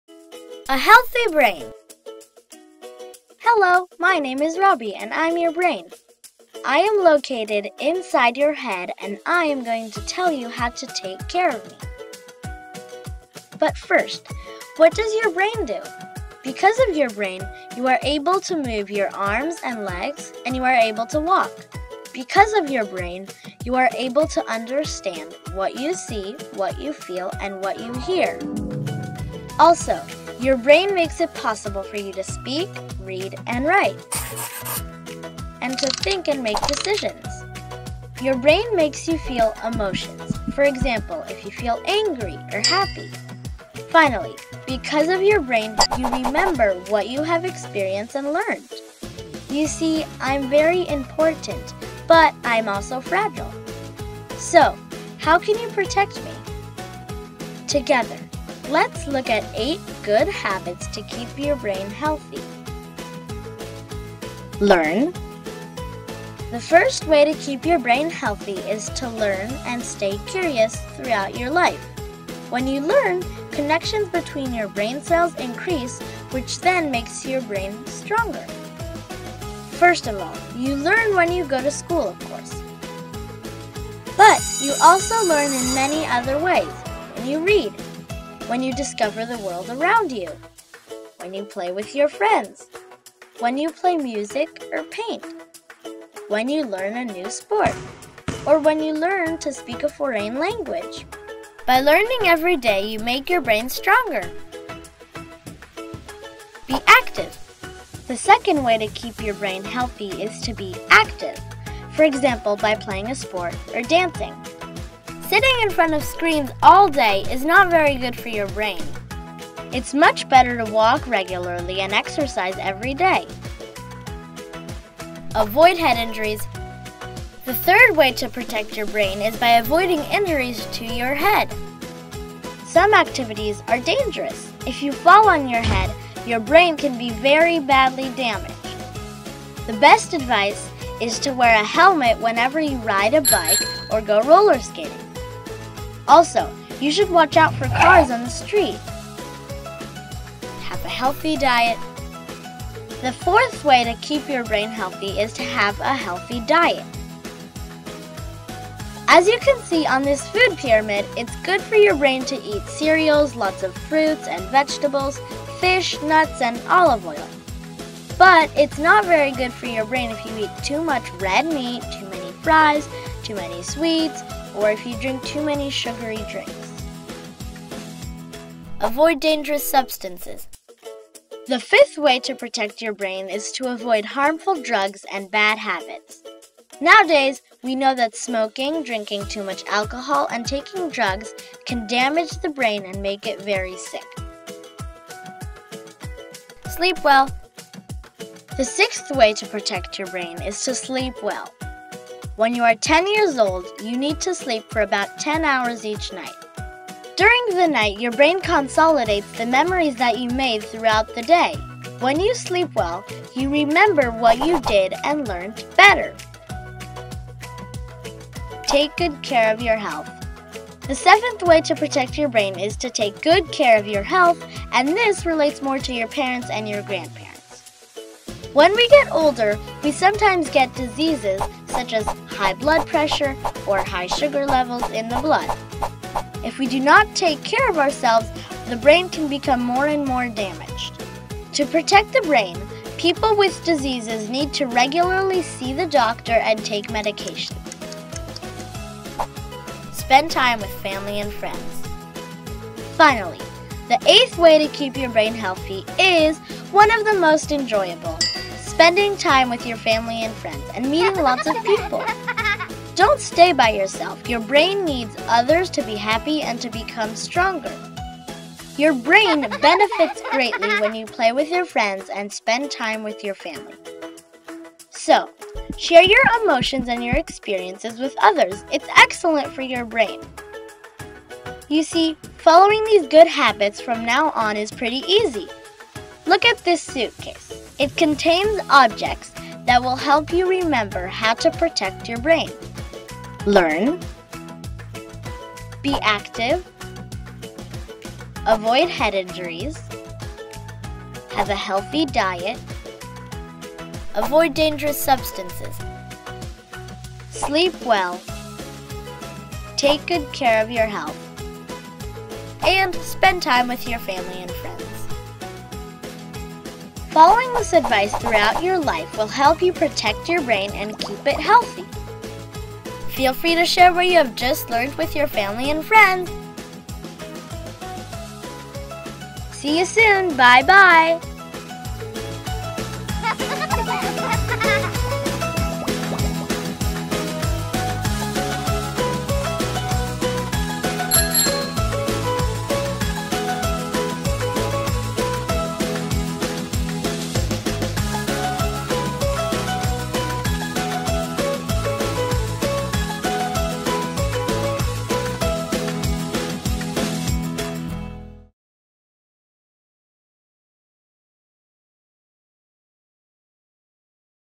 MyBrainRobbie.org (English version): A HEALTHY BRAIN !
The present initiative is a creative effort to promote better understanding of brain health in children aged 6 to 12 years old and, more broadly, to increase global public awareness of the importance of brain health to all age groups. This project has been supported by the Alzheimer’s Association and the Global Brain Health Institute.